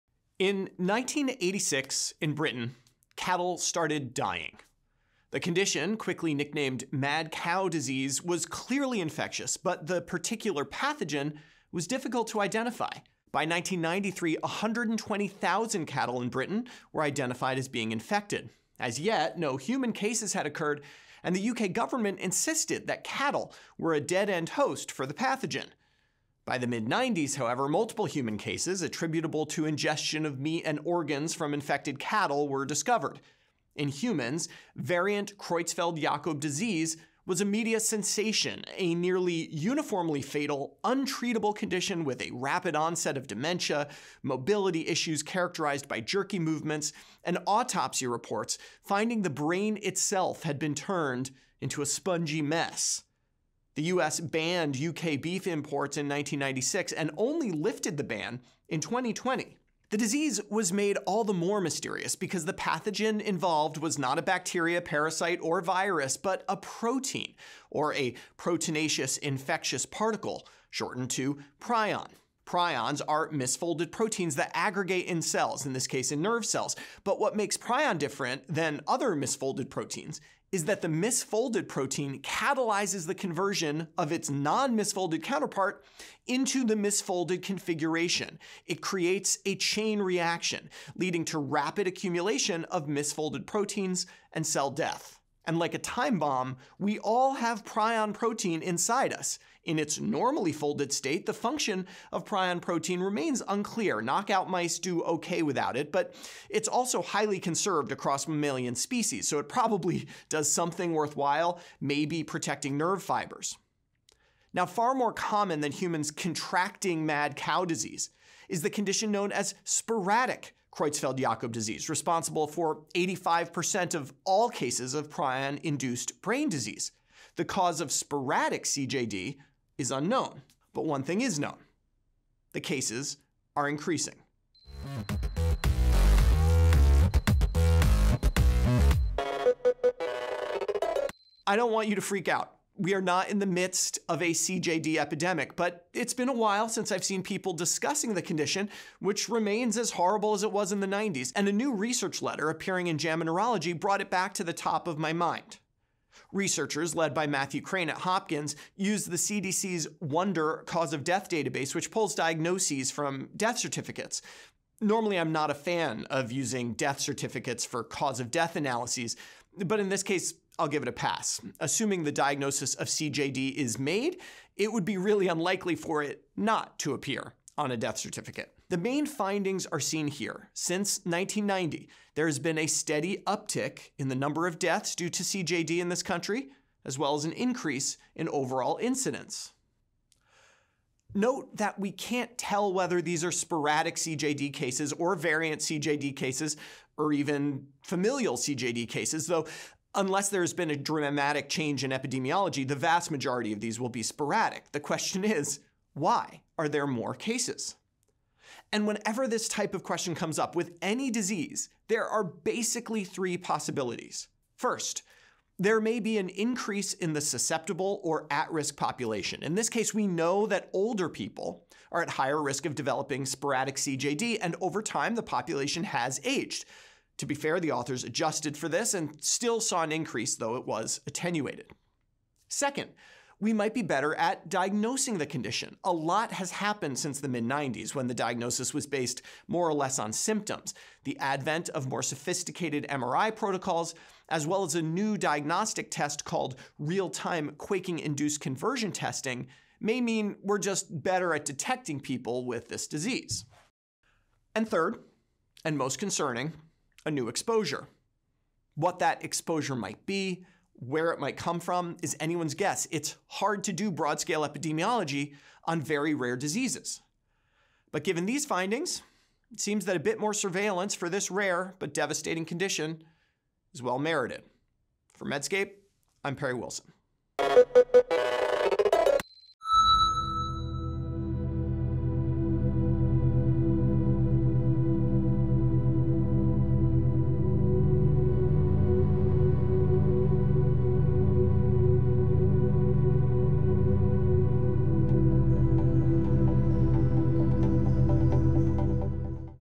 Why Are Prion Diseases on the Rise?
Remember the outbreak of "mad cow disease" in the 1990s? Creutzfeldt-Jakob is increasing again, but this time, we don't know why.

-- TRANSCRIPT --
In 1986, in Britain, cattle started dying.

The condition, quickly nicknamed "mad cow disease," was clearly infectious, but the particular pathogen was difficult to identify. By 1993, 120,000 cattle in Britain were identified as being infected. As yet, no human cases had occurred and the UK government insisted that cattle were a dead-end host for the pathogen. By the mid-1990s, however, multiple human cases, attributable to ingestion of meat and organs from infected cattle, were discovered. In humans, variant Creutzfeldt-Jakob disease (CJD) was a media sensation — a nearly uniformly fatal, untreatable condition with a rapid onset of dementia, mobility issues characterized by jerky movements, and autopsy reports finding that the brain itself had turned into a spongy mess.

The United States banned UK beef imports in 1996 and only lifted the ban in 2020.

The disease was made all the more mysterious because the pathogen involved was not a bacterium, parasite, or virus, but a protein — or a proteinaceous infectious particle, shortened to "prion."

Prions are misfolded proteins that aggregate in cells — in this case, in nerve cells. But what makes prions different from other misfolded proteins is that the misfolded protein catalyzes the conversion of its non-misfolded counterpart into the misfolded configuration. It creates a chain reaction, leading to rapid accumulation of misfolded proteins and cell death.

And, like a time bomb, we all have prion protein inside us. In its normally folded state, the function of prion protein remains unclear — knockout mice do okay without it — but it is also highly conserved across mammalian species, so it probably does something worthwhile, perhaps protecting nerve fibers.

Far more common than humans contracting mad cow disease is the condition known as sporadic CJD, responsible for 85% of all cases of prion-induced brain disease. The cause of sporadic CJD is unknown.

But one thing is known: Cases are increasing.

I don't want you to freak out; we are not in the midst of a CJD epidemic. But it's been a while since I've seen people discussing the condition — which remains as horrible as it was in the 1990s — and a new research letter appearing in JAMA Neurology brought it back to the top of my mind.

Researchers, led by Matthew Crane at Hopkins, used the CDC's WONDER cause-of-death database, which pulls diagnoses from death certificates. Normally, I'm not a fan of using death certificates for cause-of-death analyses, but in this case I'll give it a pass. Assuming that the diagnosis of CJD is made, it would be really unlikely for it not to appear on a death certificate.

The main findings are seen here. Since 1990, there has been a steady uptick in the number of deaths due to CJD in this country, as well as an increase in overall incidence.

Note that we can't tell whether these are sporadic CJD cases or variant CJD cases or even familial CJD cases; however, unless there has been a dramatic change in epidemiology, the vast majority of these will be sporadic.

The question is, why are there more cases?

Whenever this type of question comes up with any disease, there are basically three possibilities:

First, there may be an increase in the susceptible, or at-risk, population. In this case, we know that older people are at higher risk of developing sporadic CJD, and over time, the population has aged. To be fair, the authors adjusted for this and still saw an increase, though it was attenuated.

Second, we might be better at diagnosing the condition. A lot has happened since the mid-1990s, when the diagnosis was based more or less on symptoms. The advent of more sophisticated MRI protocols as well as a new diagnostic test called "real-time quaking-induced conversion testing" may mean we are just better at detecting people with this disease.

Third (and most concerning), a new exposure has occurred. What that exposure might be, where it might come from, is anyone's guess. It's hard to do broad-scale epidemiology on very rare diseases.

But given these findings, it seems that a bit more surveillance for this rare but devastating condition is well merited.